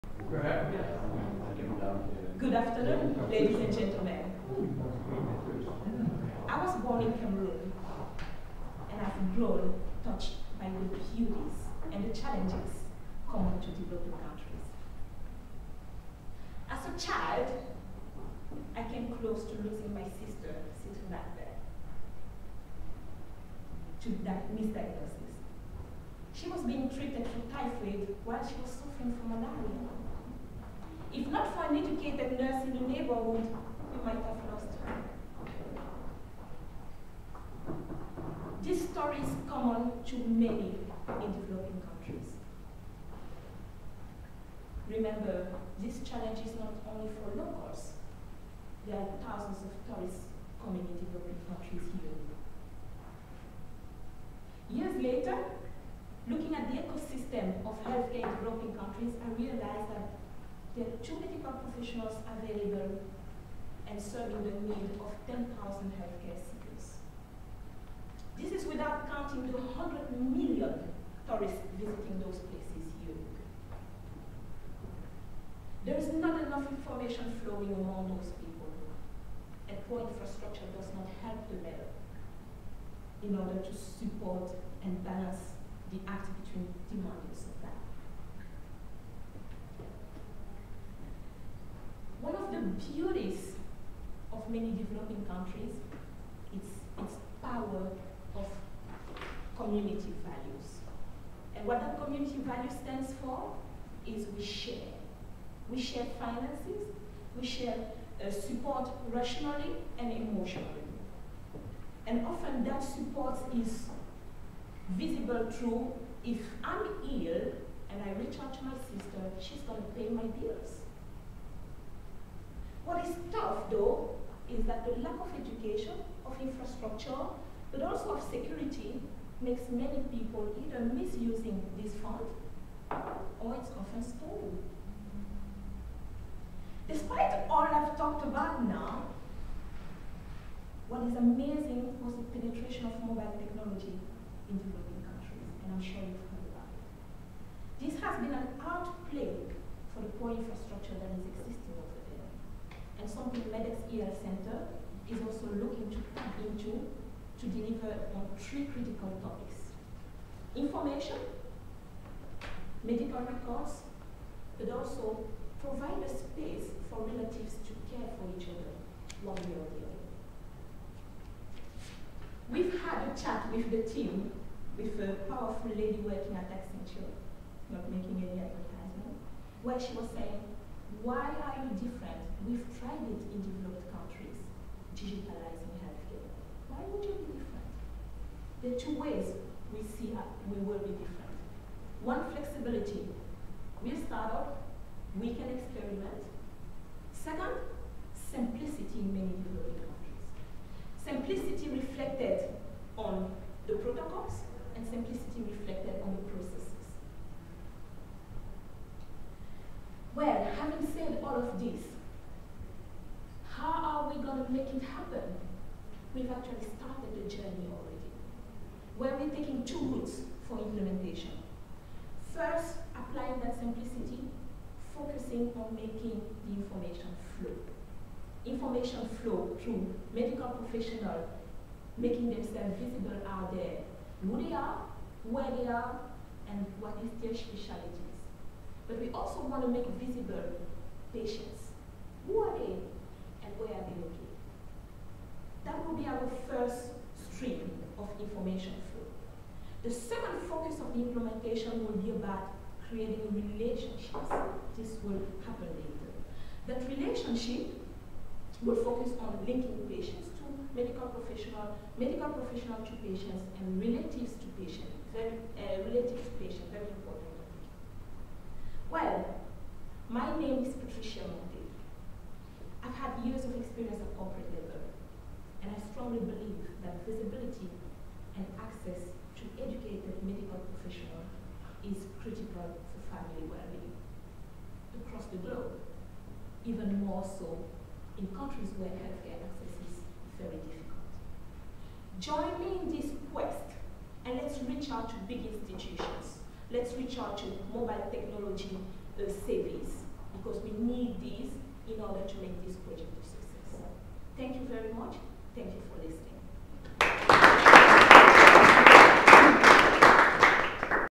Digital health for and in developing countries- pitch to the TEDx Amsterdam jury
Pitch by Patricia Monthe to the TEDx Amsterdam jury
on Fri, Oct 17th 2014